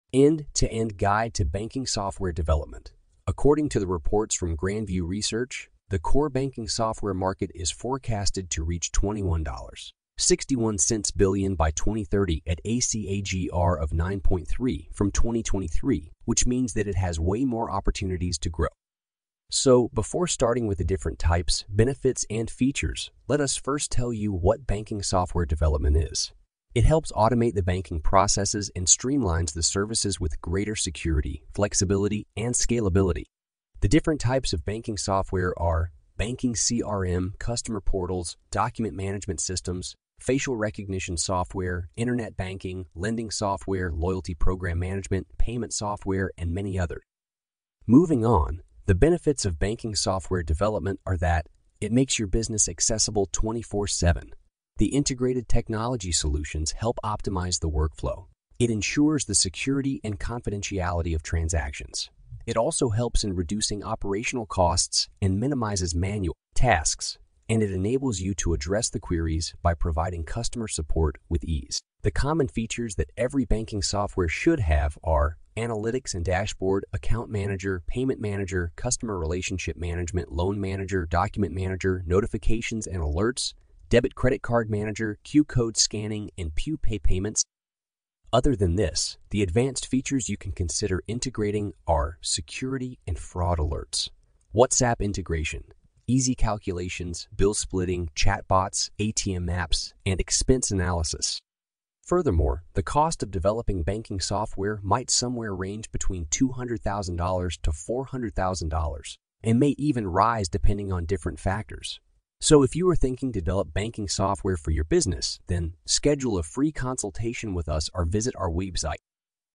What is Banking Software Development | Types | Benefits | Features | Cost | In just 2 Minutes
The end-to-end guide to banking software development | Whether you're a student or a programmer every detail is included in the video.